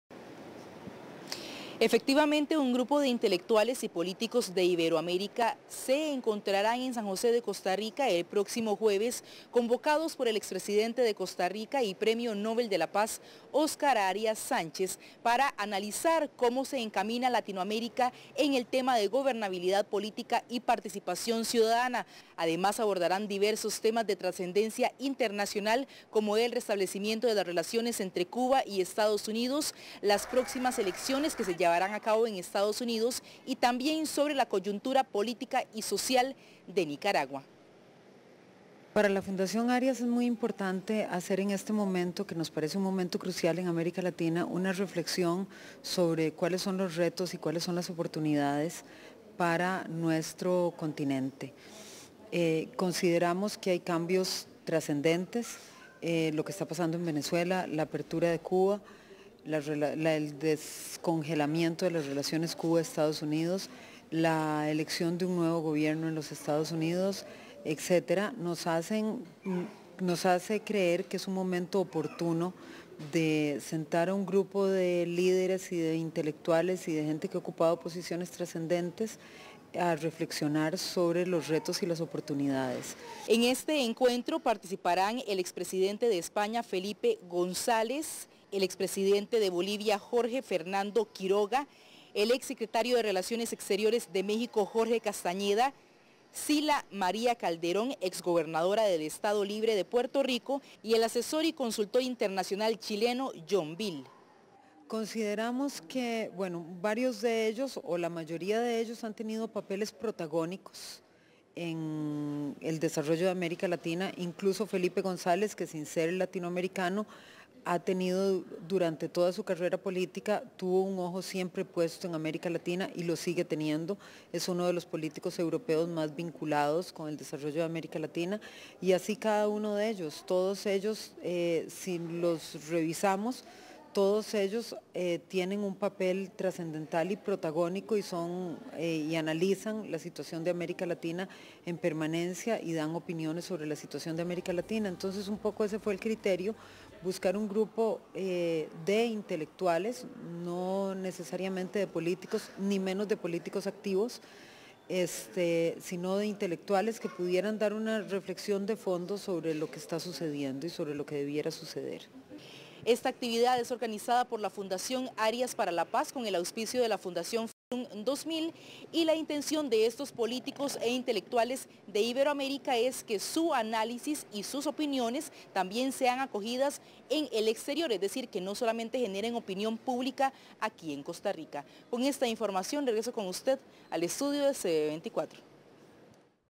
Costa Rica: Expresidentes e intelectuales discutirán sobre actualidad y futuro de América Latina
El exmandatario de Costa Rica y premio Nobel de la Paz, Óscar Arias y el expresidentedel gobierno de España,  Felipe González junto a otros intelectuales de Iberoamérica, analizarán temas de trascendencia internacional como el acercamiento entre Cuba y Estados Unidos, así como la coyuntura actual que vive Nicaragua.
Video es un extracto del noticiero de CB24 Noticias de la 1:00 de la tarde hora Centroamérica, 2:00 de la tarde hora Panamá.

Angélica López/Corresponsal Costa Rica/CB24